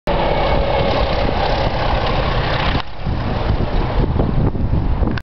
Trains through kettering 09/04/09 part 2 network rail 31465
Network Rail 31465 growls through Kettering light engine.